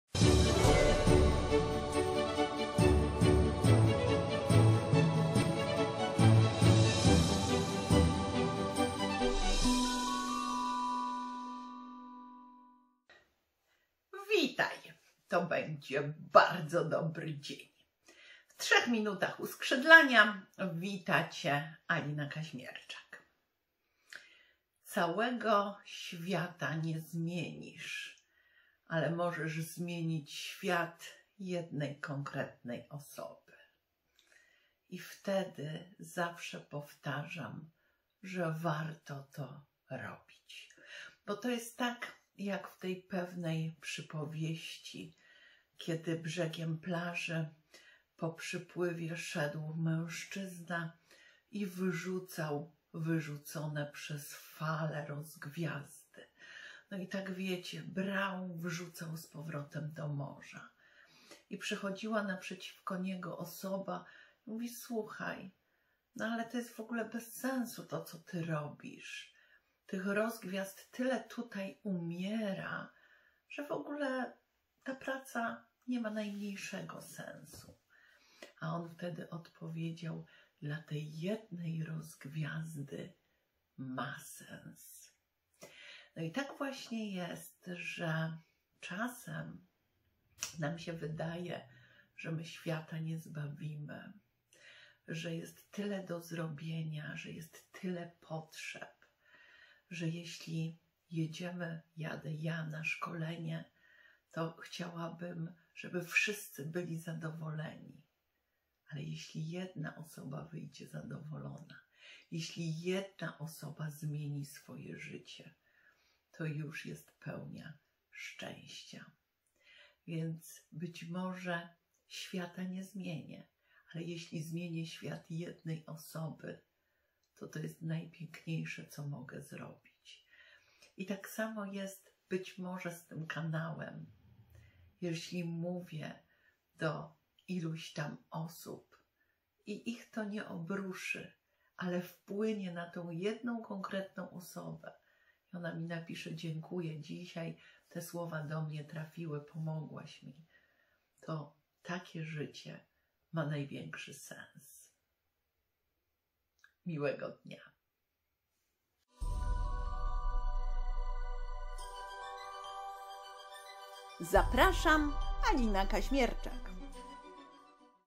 Całego świata nie zmienisz. Przypowieść o rozgwiazdach
Całego świata nie zmienisz, ale możesz zmienić świat jednej osoby. Wtedy wiedz, że warto po to żyć!